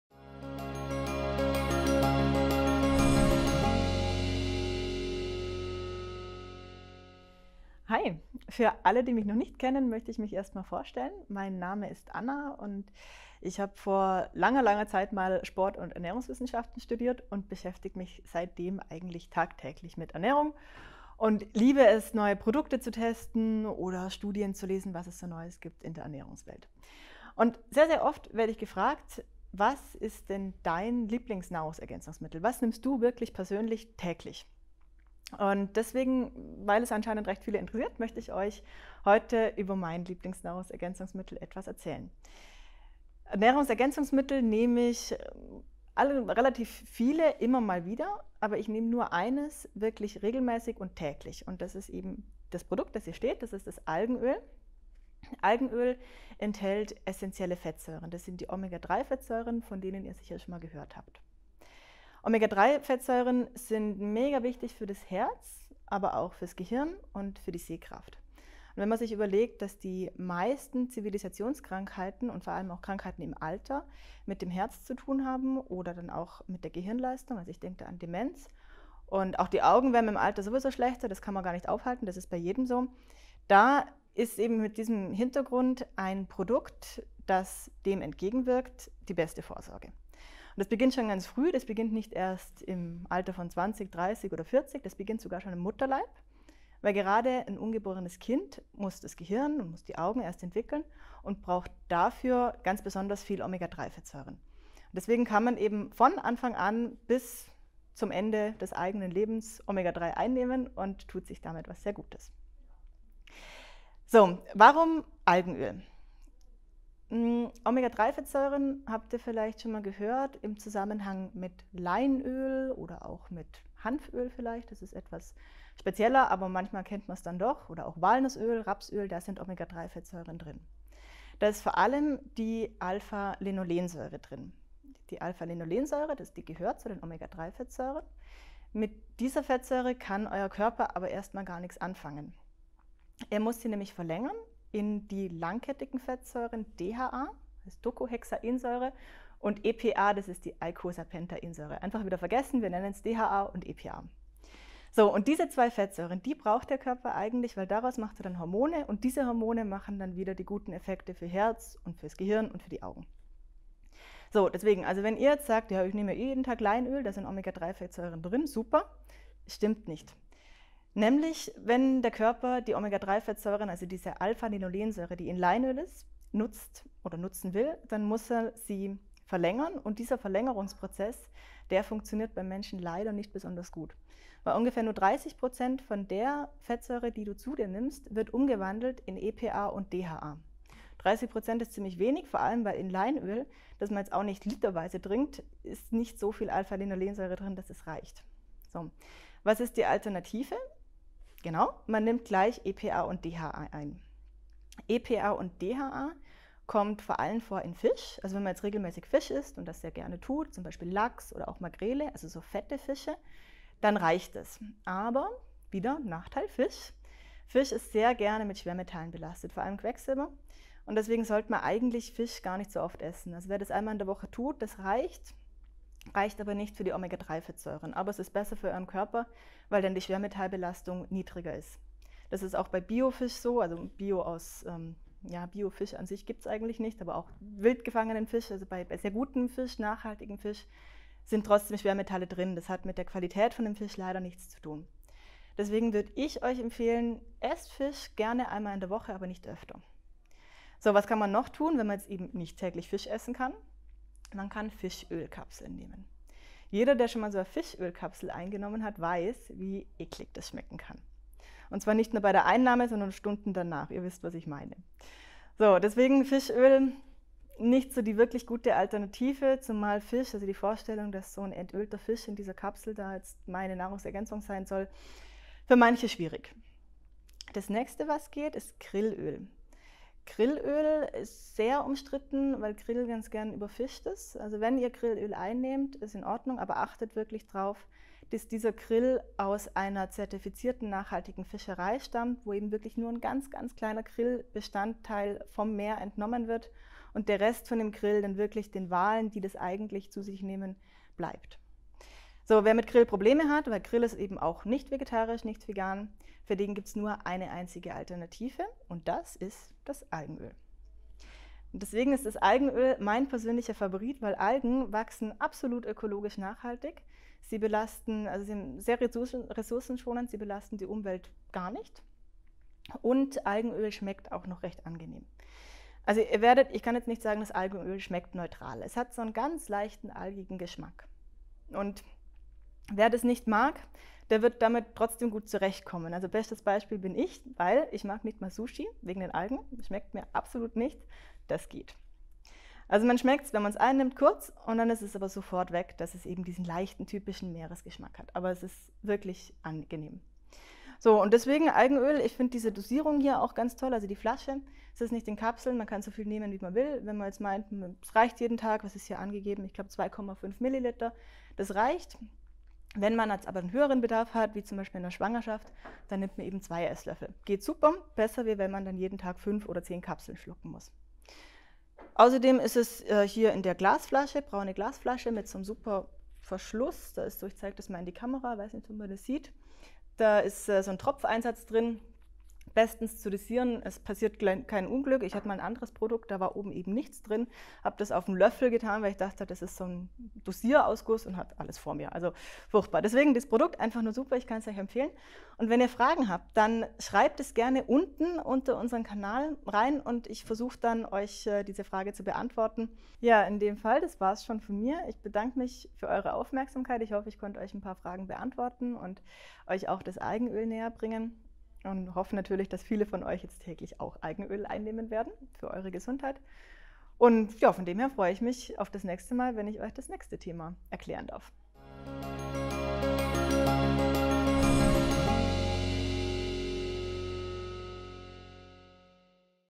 Omega 3 | Omega 3 Fettsäuren und ihre Wirkung - das musst du wissen | myFairtrade
Omega-3-Fettsäuren sind elementar für die Gesundheit: Sie tragen zu einer normalen Gehirn-, Herz- und Sehfunktion bei. Unsere Produktmanagerin Anna beantwortet die wichtigsten Fragen rund um das Thema Omega 3 und stellt ihr Lieblingsprodukt flüssiges Omega 3 aus Algenöl vor.

Erfahre in diesem Video:

-Was sind Omega 3, Alpha-Linolensäure (ALA), EPA und DHA und was der Zusammenhang ist
- In welchen Lebensmitteln Omega 3 steckt
- Wie du deinen täglichen Bedarf an Omega 3 decken kannst & warum Fisch nicht die beste Wahl dafür ist